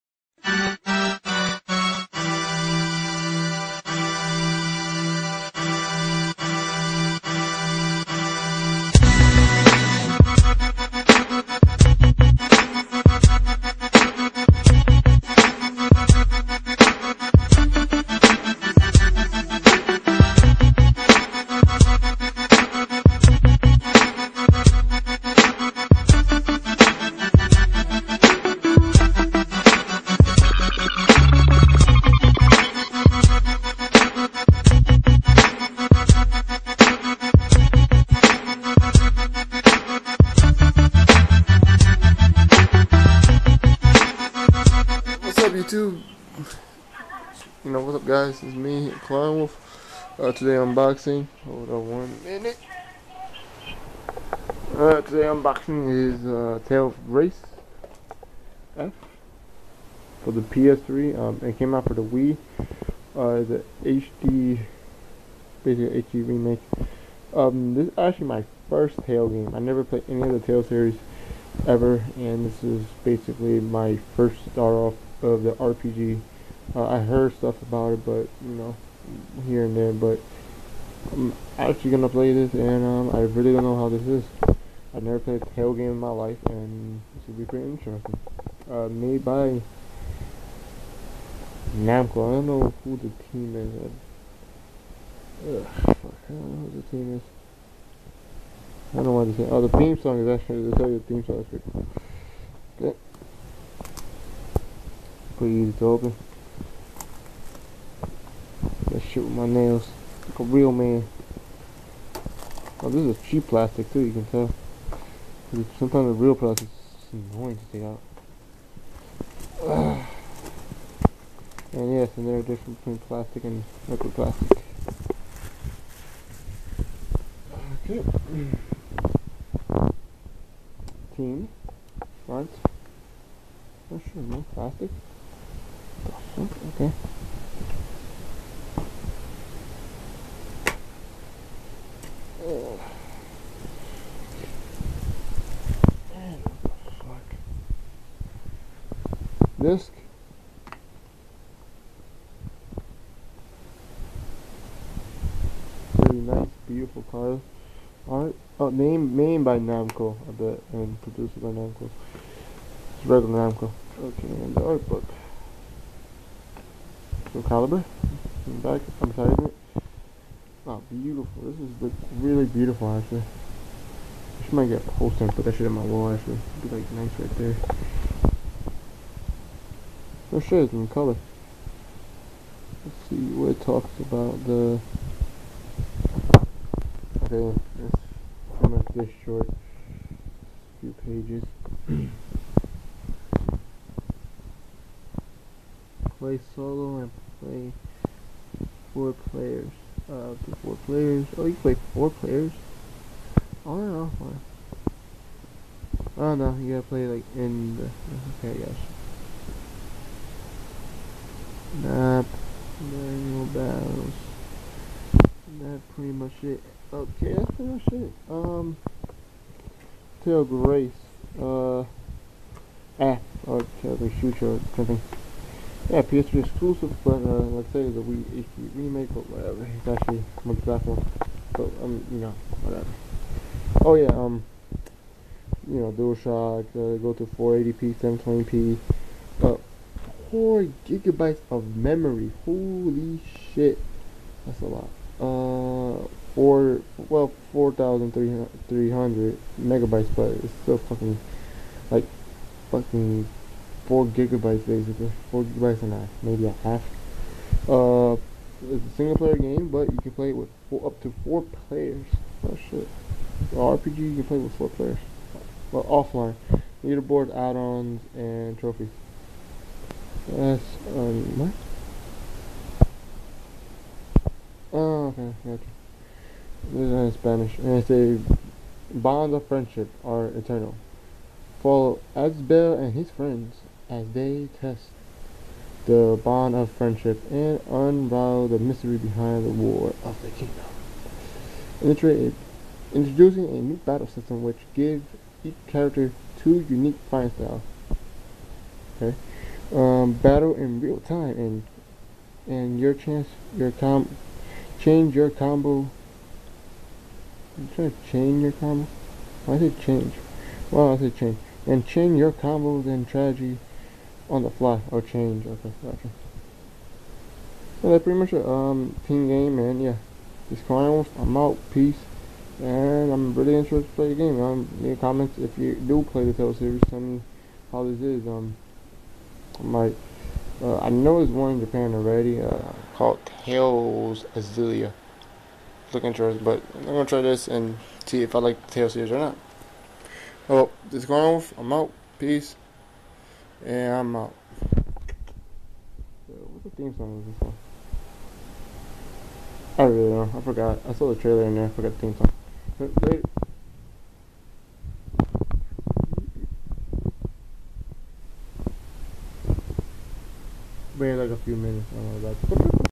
Tales of Graces F Unboxing
This Is My First Tales Game Ever.Time To Enjoy A Epic Story Of Friendship In Tales of Graces ƒ.Developer Is Namco Tales Studio And The Publisher Is Namco Bandai.Enjoy